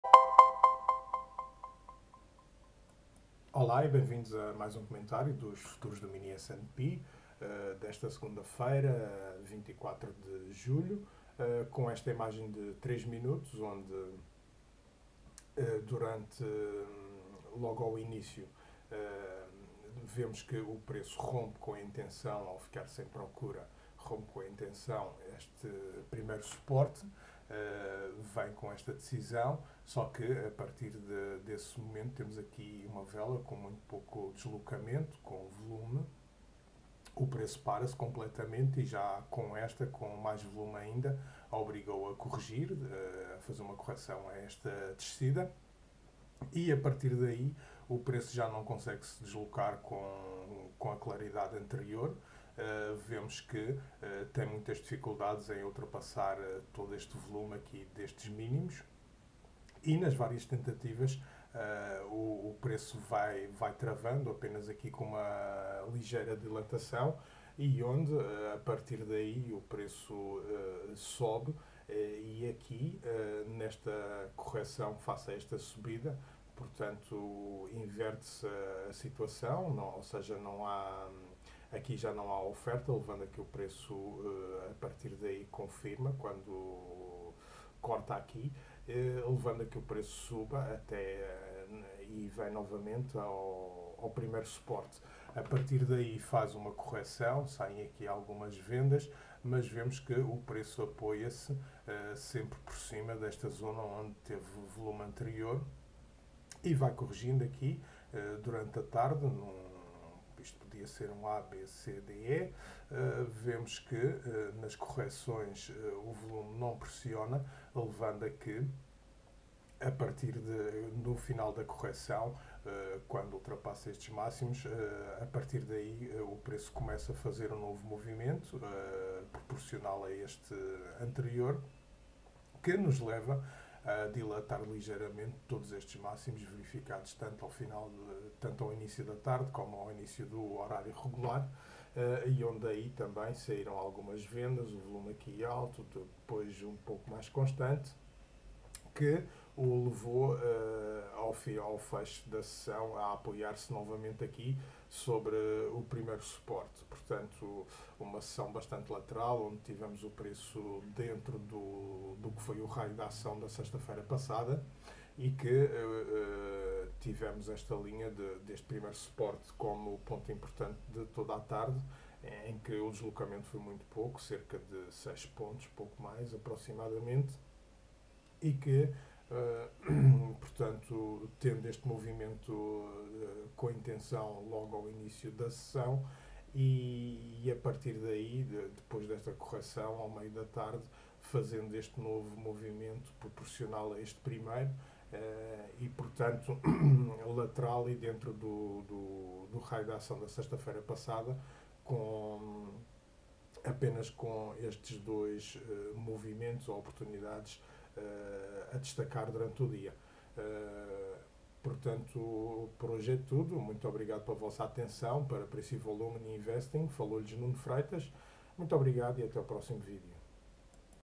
Primeiro nível de suporte sempre presente
O mini S&P 500 continua a descansar da última subida que leva, e vai consumindo tempo dentro dos mesmos dígitos do dia anterior. Ao inicio rompe com intenção o primeiro suporte, mas as compras pararam a descida levando a que recuperasse o suporto. Depois de corrigir sobre essa zona, efectua um novo movimento ascendente proporcional ao primeiro que o leva aos máximos do inicio da tarde, para depois terminar o dia na zona que foi referência toda a tarde. La operativa en acciones, futuros y forex contiene un riesgo sustancial y no es para todos los inversores. Lee en el siguiente enlace la divulgación completa de riesgos: